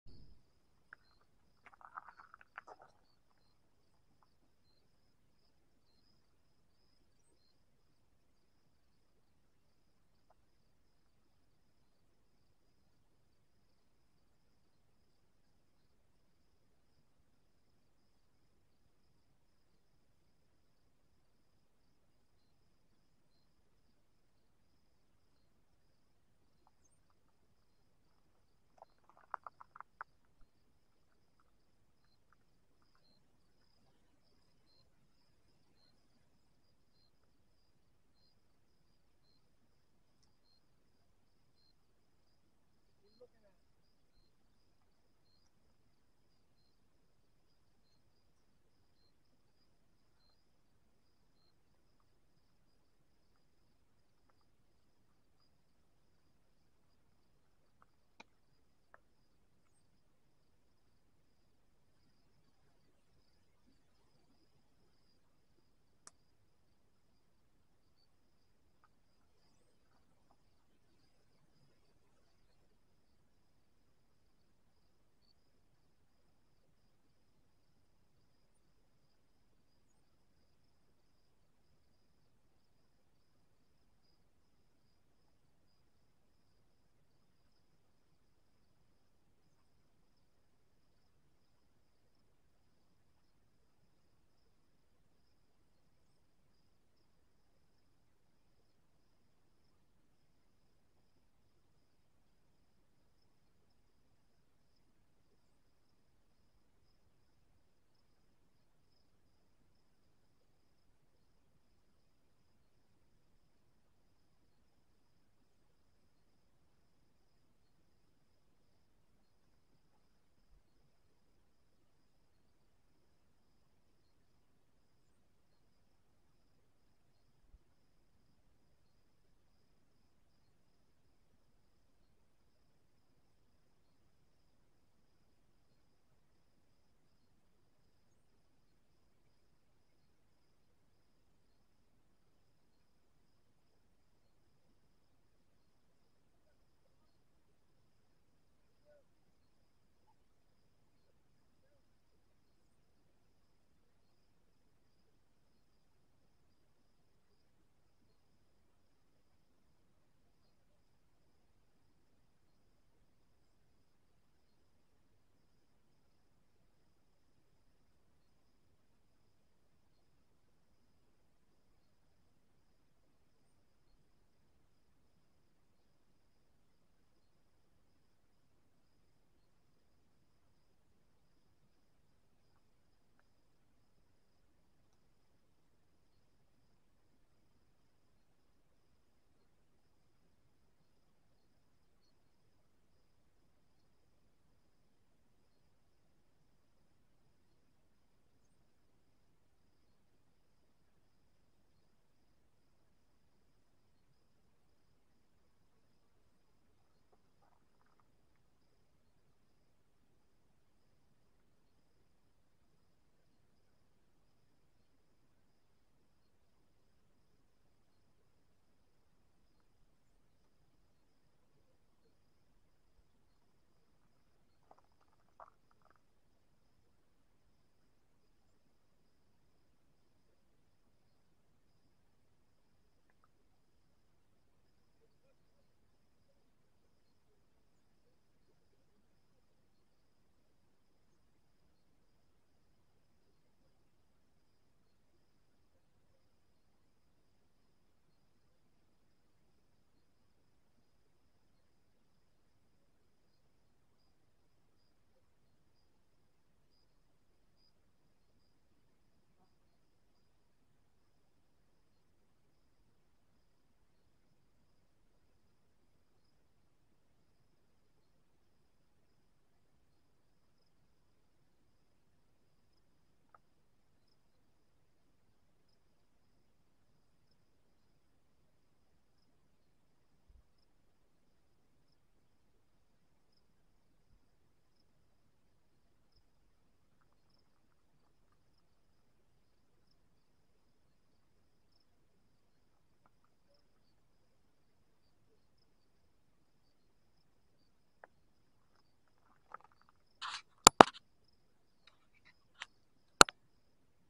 Wheat field 2024
Watch as the birds come and go from this 17-acre wheat field located on the Cibola National Wildlife Refuge Island Unit. Sadly, the camera was not able to capture all of the birds in the back of the field.